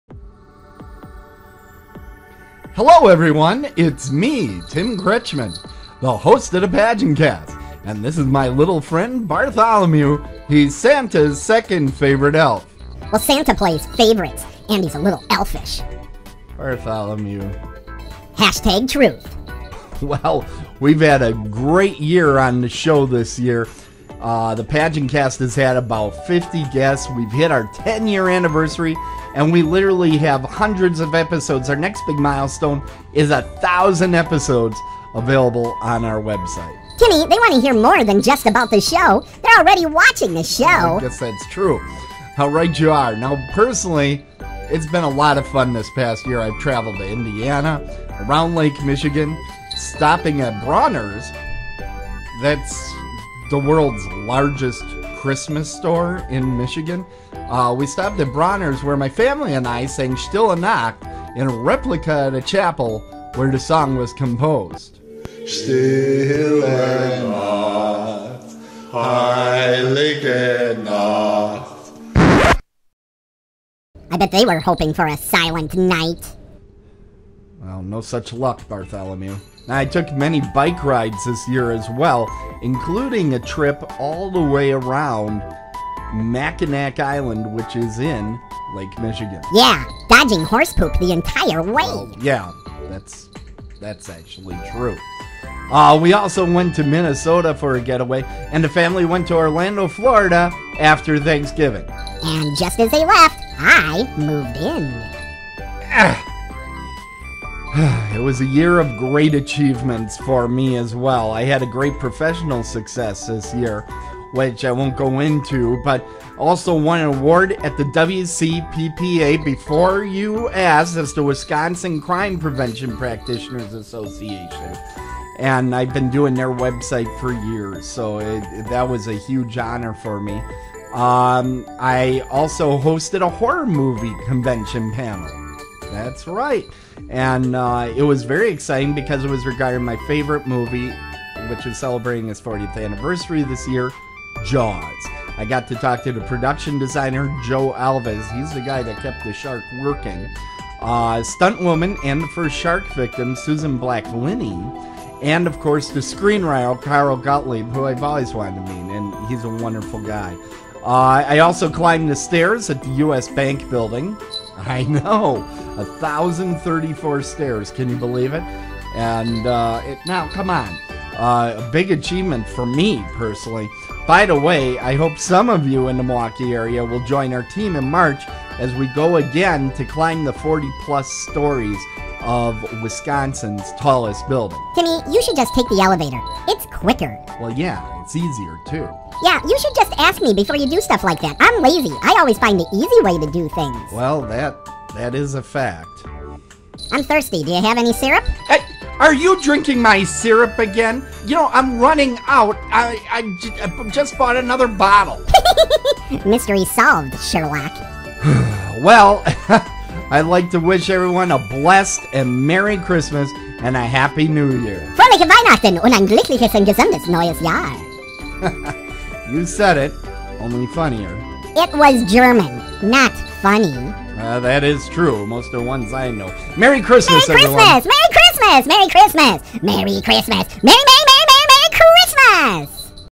Tim & Bartholomew's 2015 Christmas Greeting
Tim Kretschmann of the PageantCast and Bartholomew, Santa's Second Favorite Elf, wish their viewers a very Merry Christmas and a Happy New Year!